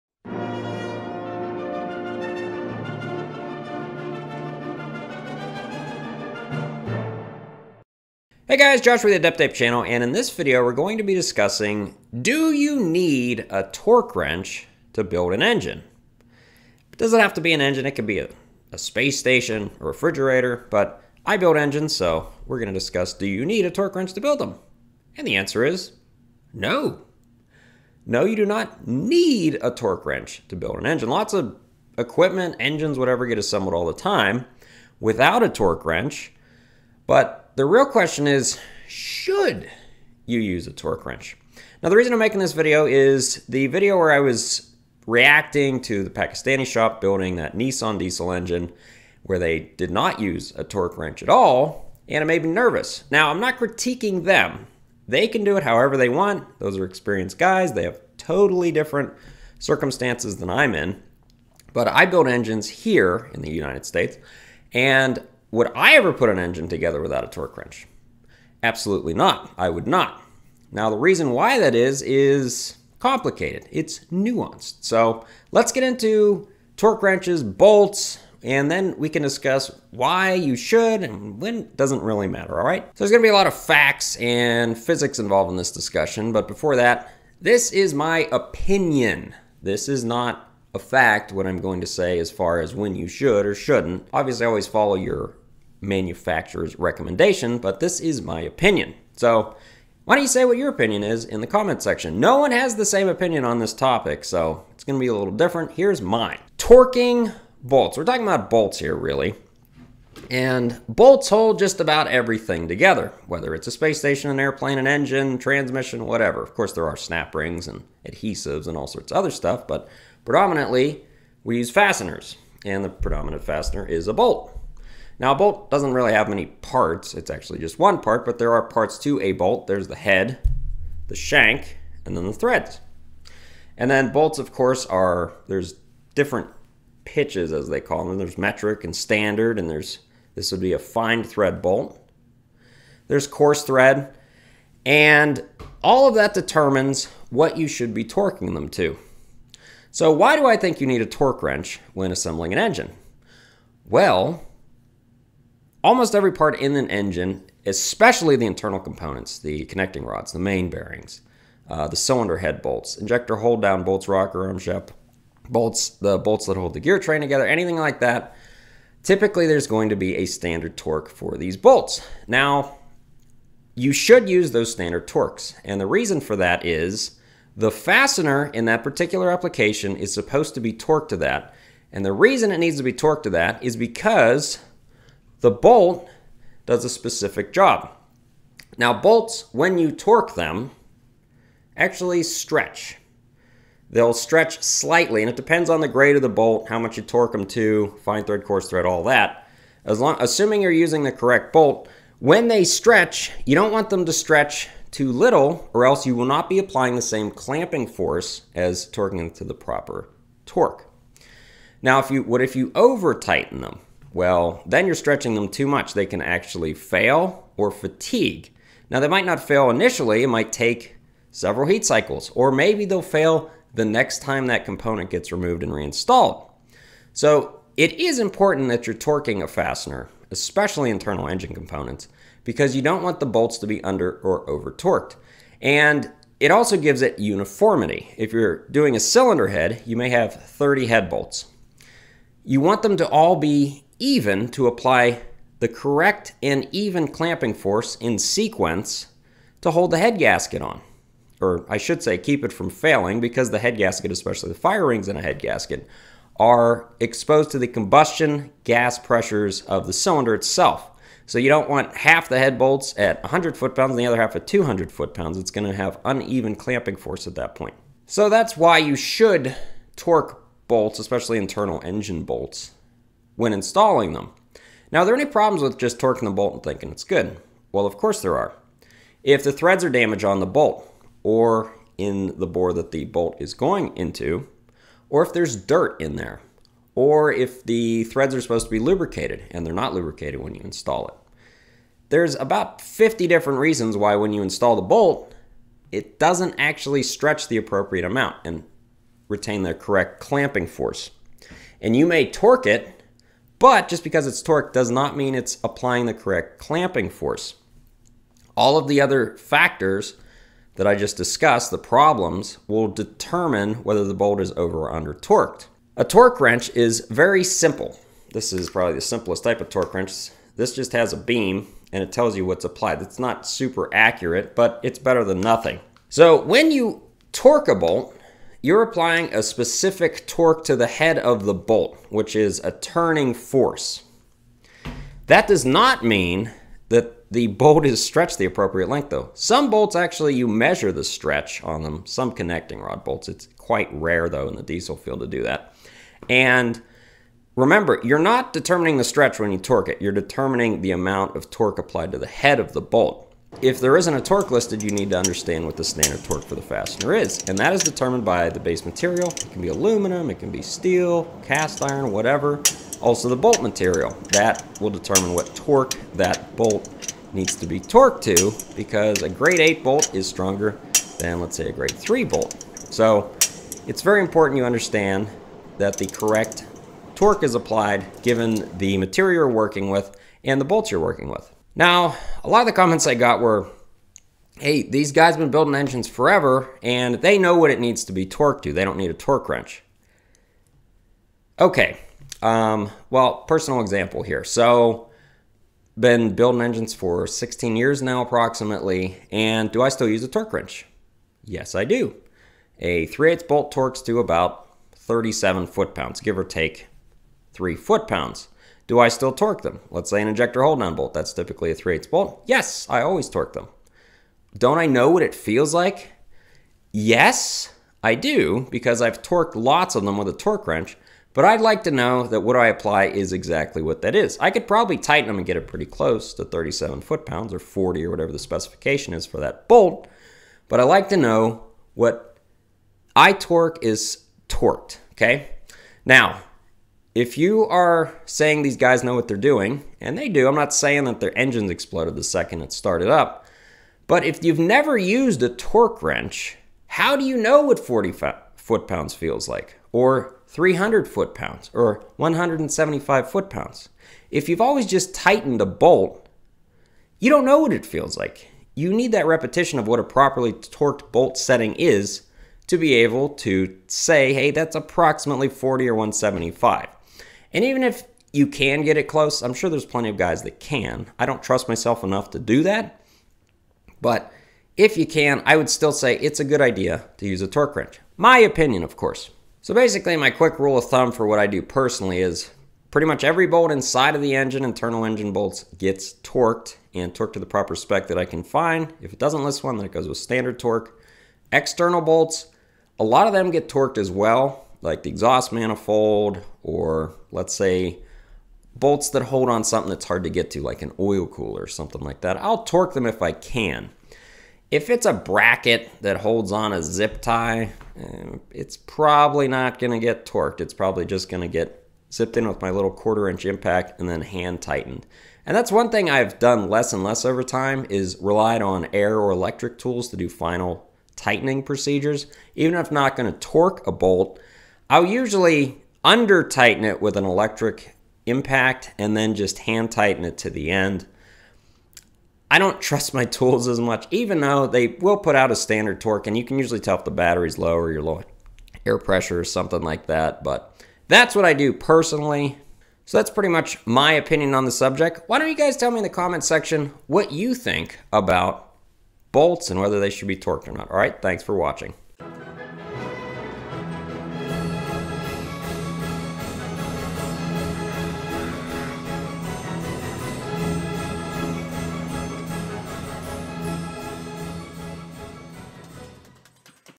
Do You Need a Torque Wrench to Fix an Engine?
In this video we are discussing whether a Torque Wrench is required to fix or build an engine. What are your thoughts?
Precision Instruments 1/2" Torque Wrench
Precision Instruments 3/8" Torque Wrench